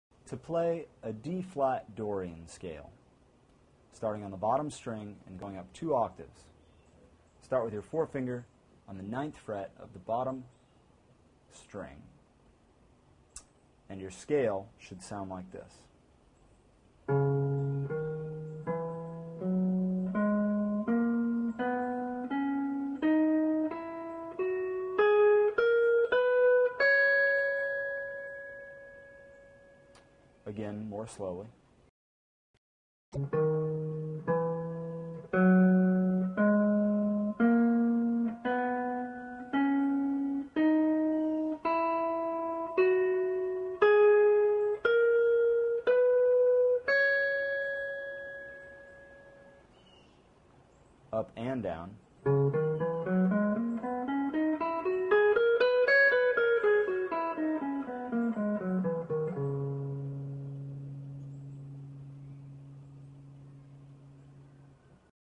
How to Play the Db (D-Flat) Dorian Scale on Guitar
How to Play the Db (D-Flat) Dorian Scale on Guitar. Part of the series: How to Play Scales on the Guitar 4. Learn how to play the Db (D Flat) Dorian scale on the guitar for better solos without reading sheet music.